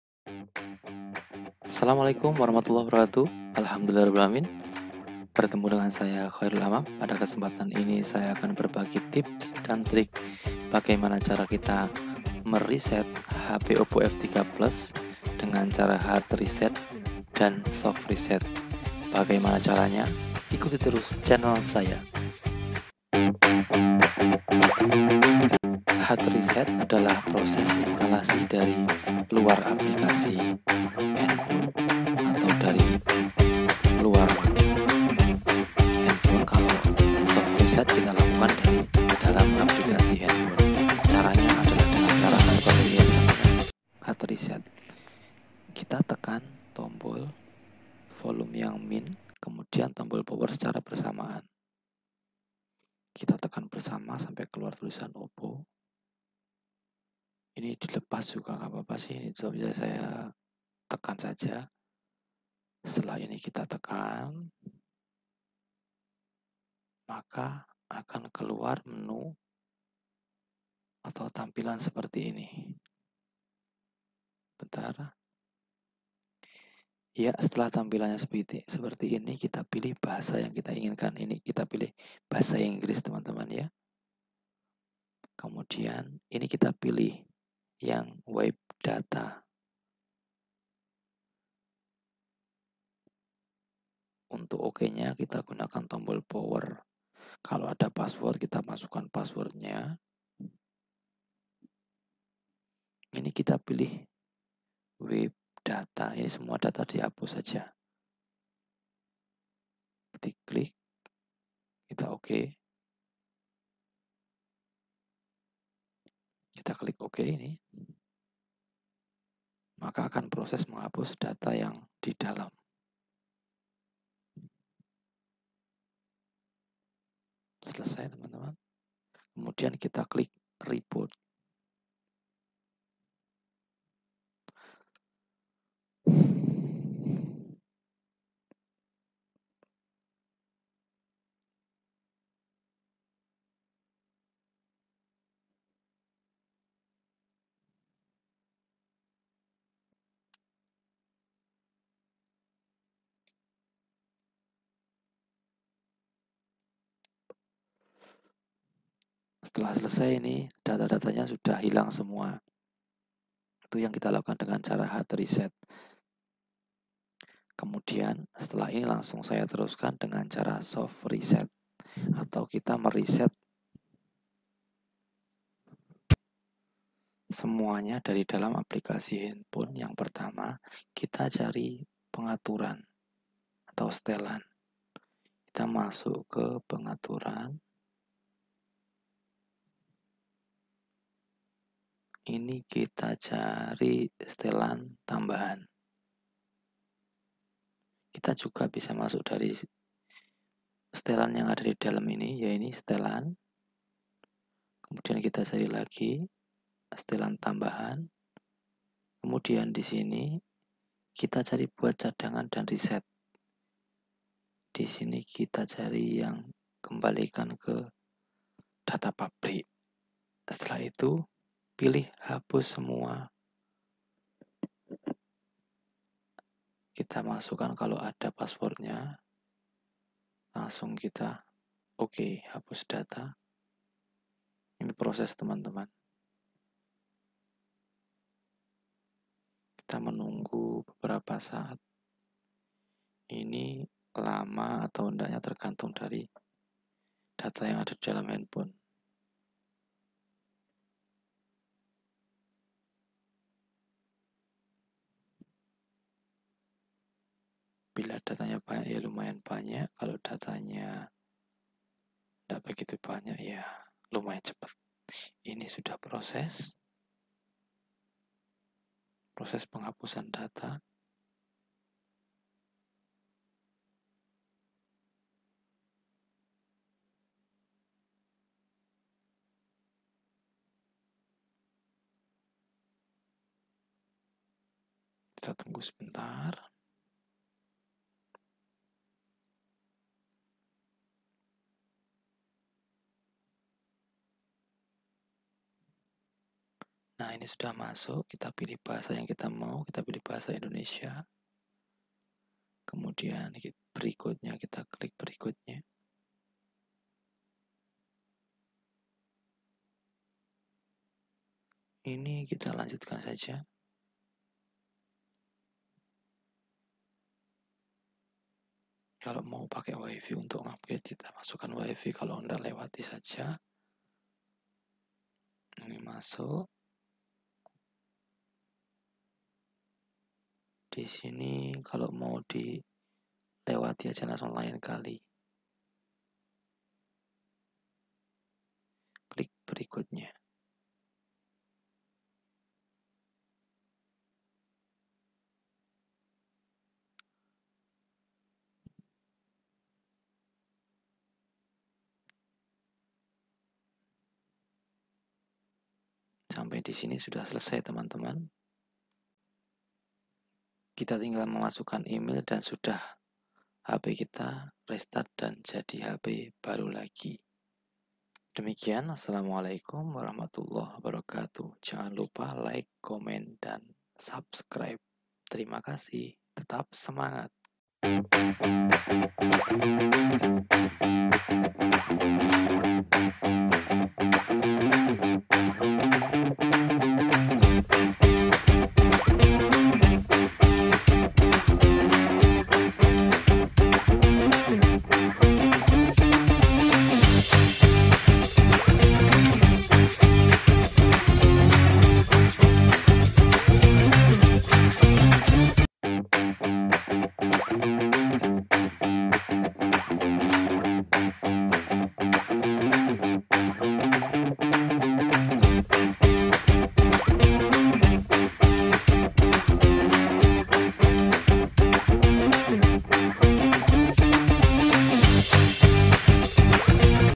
Reset OPPO F3/F3 Plus secara Hard & Soft 2020
Reset F3/F3 Plus secara Hard & Soft adalah cara flash yang dilakukan dari luar dan dari dalam aplikasi oppo F3/f3 Plus, cara ini sangat gampang meskipun sangat kecil semoga bermanfaat.
jangan lupa dukung supaya lebih berkembang.
saya bakal terus berusaha menyajikan video2 yang anda butuhkan mohon dukungan supaya terus berkembang dan berkarya